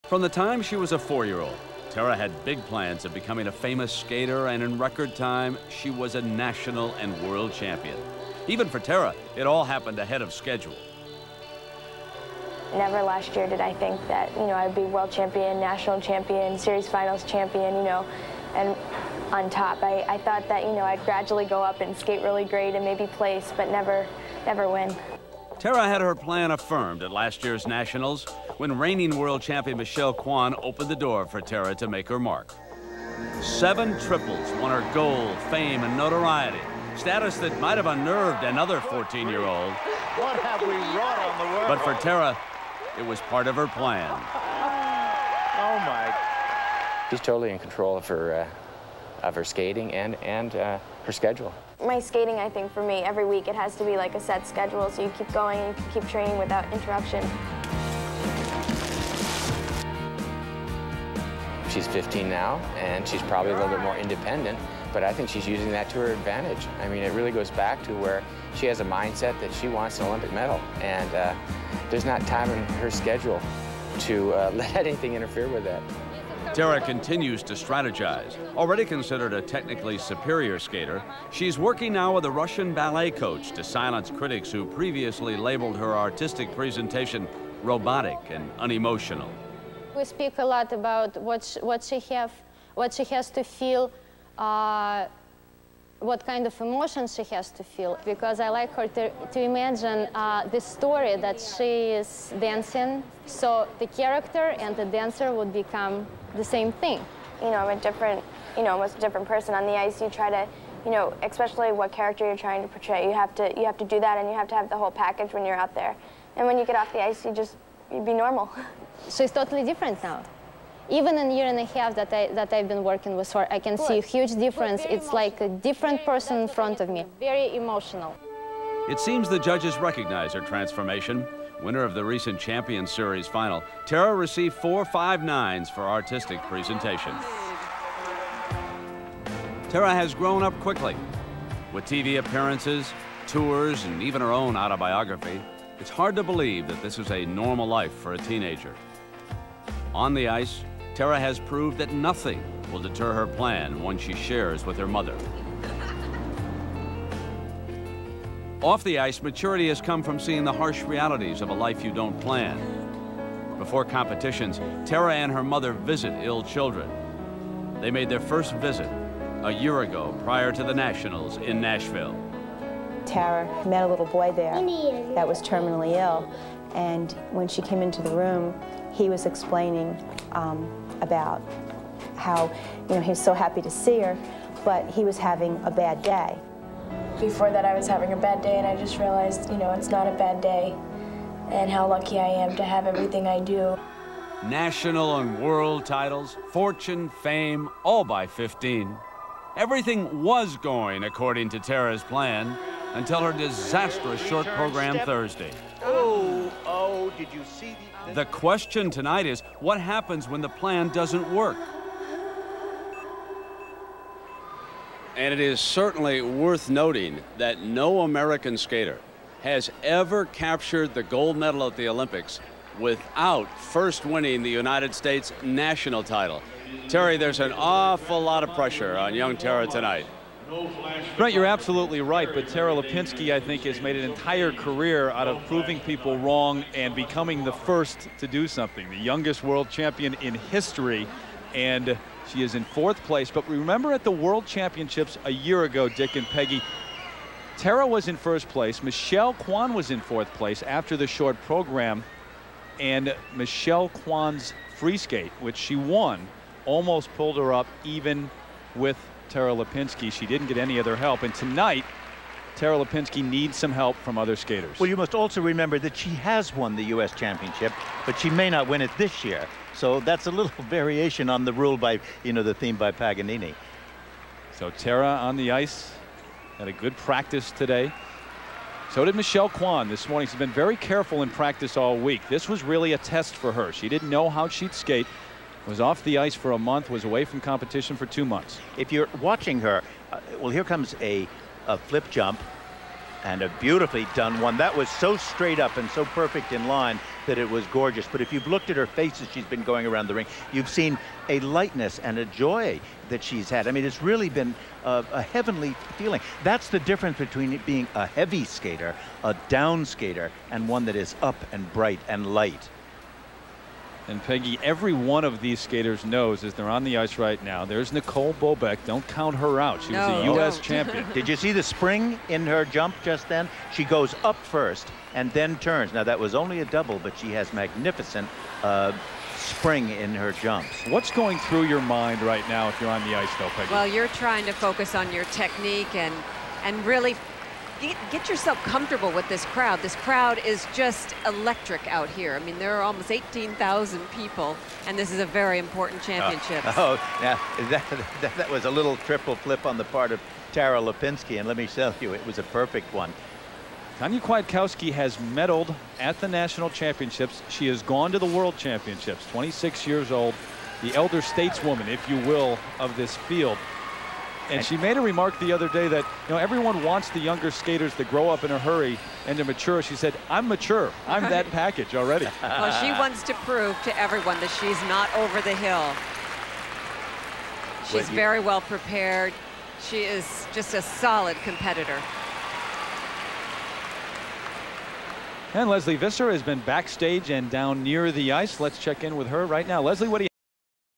Profile on Tara Lipinski - 1998 United States Figure Skating Championships, Ladies' Free Skate
Philadelphia, Pennsylvania, USA - 1998 United States Figure Skating Championships, Ladies' Free Skate - First, a profile on Tara Lipinski, followed by a portion of the 6-minute warm up of the final group of skaters.